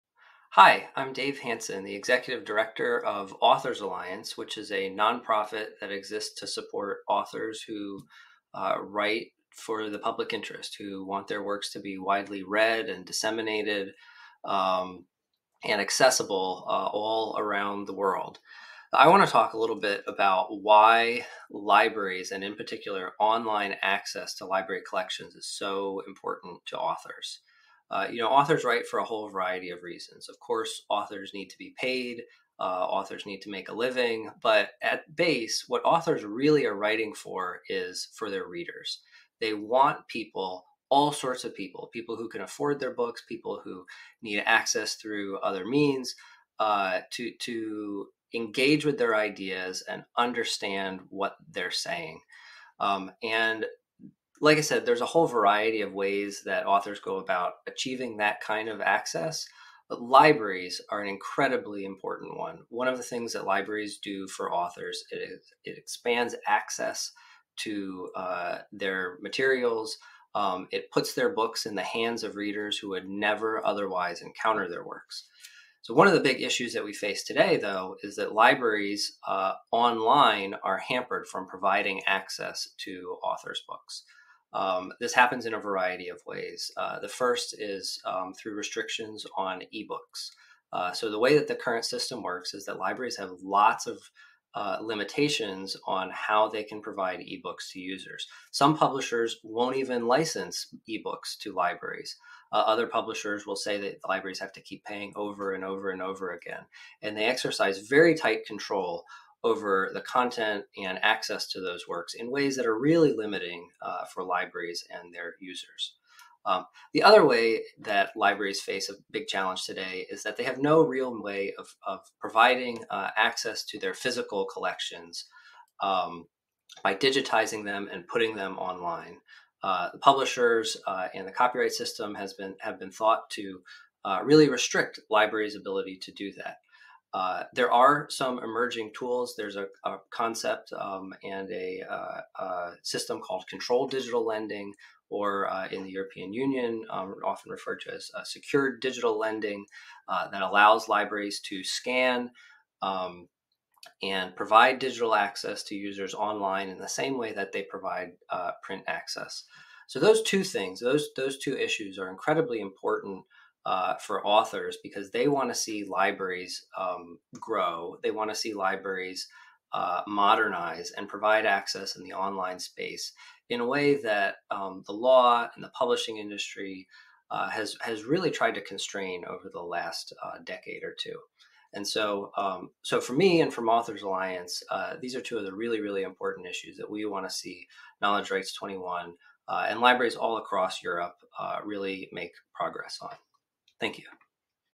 Authors & libraries: enhancing the synergies – Dave Hansen
Dave Hansen (Executive Director, Authors Alliance) explains how we can enhance the synergies between authors and libraries